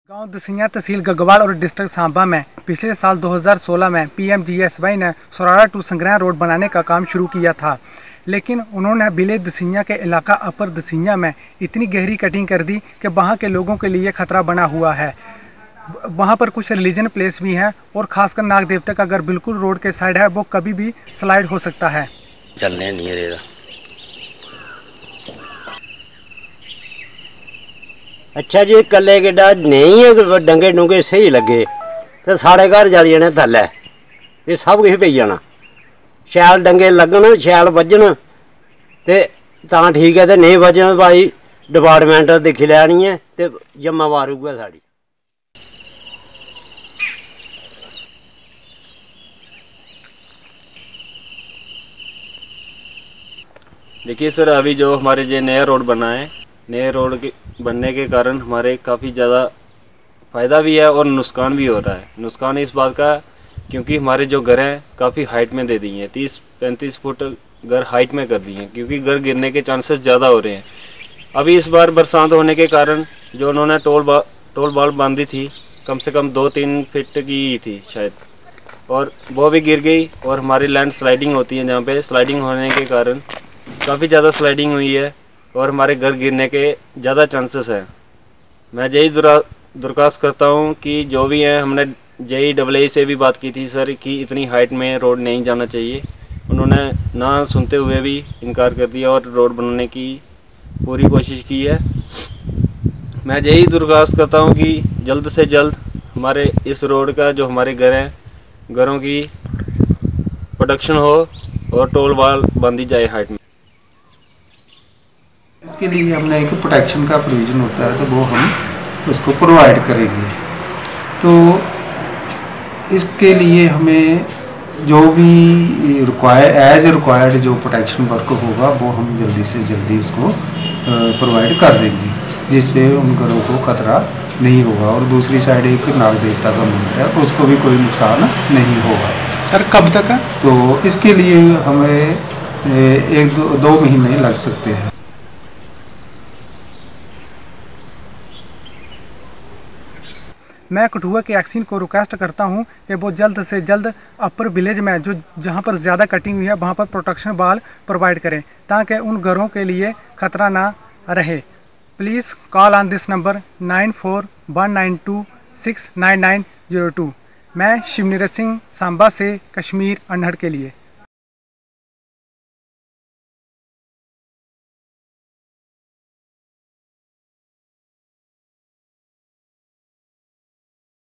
Construction of Link Road in Samba Puts Lives of Residents at Risk | Shiv Niraz reports
Shiv Niraz reports from Samba, Jammu and Kashmir for Video Volunteers.

In village Dessian, Samba due to construction of link road from Surara to Sygran houses of residents living on upper reaches are on risk. Village residents demand protection wall. The houses are put to risk, during the rains it creates landslides and can damage properties of locals - it is even threat to local population.